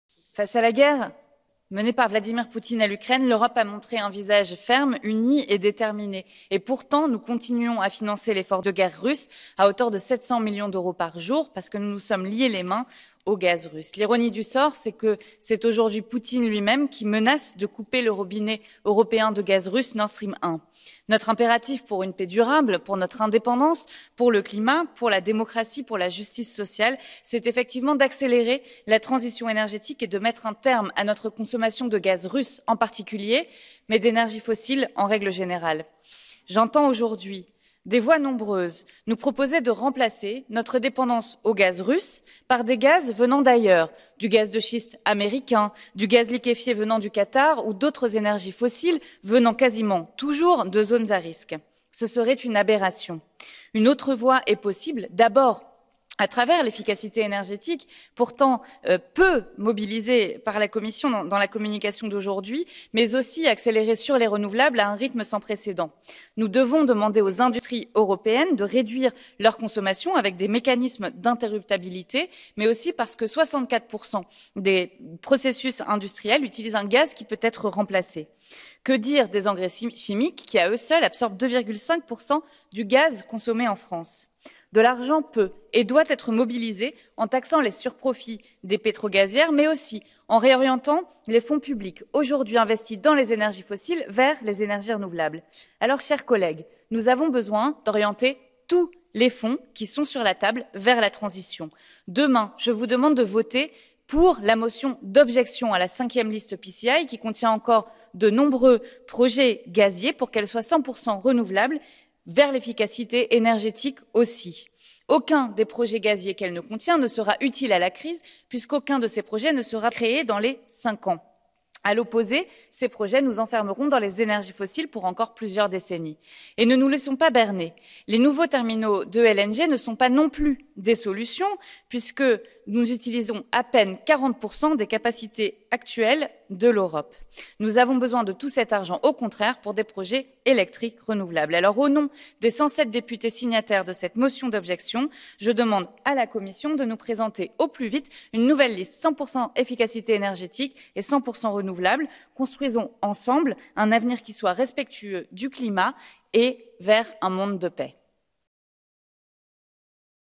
Marie Toussaint sur la nouvelle liste de Projets d'intérêt commun (PIC)
Mardi 8 mars, Marie Toussaint est intervenue sur la nouvelle liste de Projets d'intérêt commun (PIC).

Son intervention ⬇️

"Face à la guerre menée par Vladimir Poutine à l'Ukraine, l'Europe a montré un visage ferme, uni et déterminé. Et pourtant, nous continuons à financer l'effort de guerre russe à hauteur de 700 millions d'euros par jour parce que nous nous sommes lié les mains au gaz russe.

L'ironie du sort, c'est que c'est aujourd'hui Poutine lui-même qui menace de couper le robinet européen de gaz russe, Nord Stream un. Notre impératif pour une paix durable, pour notre indépendance, pour le climat, pour la démocratie, pour la justice sociale, c'est, effectivement, d'accélérer la transition énergétique et de mettre un terme à notre consommation de gaz russe, en particulier, mais d'énergies fossiles en règle générale.

J'entends aujourd'hui des voix nombreuses nous proposer de remplacer notre dépendance au gaz russe par des gaz venant d'ailleurs : du gaz de schiste américain, du gaz liquéfié venant du Qatar ou d'autres énergies fossiles venant quasiment toujours de zones à risque. Ce serait une aberration. Une autre voie est possible. D'abord, à travers l'efficacité énergétique, pourtant peu mobilisée par la Commission dans la communication d'aujourd'hui, mais aussi accélérer sur les renouvelables à un rythme sans précédent.

Nous devons demander aux industries européennes de réduire leur consommation, avec des mécanismes d'interruptibilité, mais aussi parce que 64 % des processus industriels utilisent un gaz qui peut être remplacé. Que dire des engrais chimiques qui, à eux seuls, absorbent 2,5 % du gaz consommé en France ?

De l'argent peut et doit être mobilisé en taxant les surprofits des pétrogazières, mais aussi en réorientant les fonds publics aujourd'hui investis dans les énergies fossiles vers les énergies renouvelables. Alors, chers collègues, nous avons besoin d'orienter tous les fonds qui sont sur la table vers la transition.

Demain, je vous demande de voter pour la motion d'objection à la cinquième liste PCI qui contient encore de nombreux projets gaziers pour qu'elle soit 100 % renouvelable, vers l'efficacité énergétique aussi. Aucun des projets gaziers qu'elle ne contient ne sera utile à la crise puisqu'aucun de ses projets ne sera créé dans les cinq ans.

À l'opposé, ces projets nous enfermerons dans les énergies fossiles pour encore plusieurs décennies. Et ne nous laissons pas berner. Les nouveaux terminaux de LNG ne sont pas, non plus, des solutions puisque nous utilisons à peine 40 % des capacités actuelles de l'Europe.

Nous avons besoin de tout cet argent, au contraire, pour des projets électriques renouvelables. Alors, au nom des 107 députés signataires de cette motion d'objection, je demande à la Commission de nous présenter, au plus vite, une nouvelle liste 100 % efficacité énergétique et 100 % renouvelable.

Construisons, ensemble, un avenir qui soit respectueux du climat et vers un monde de paix."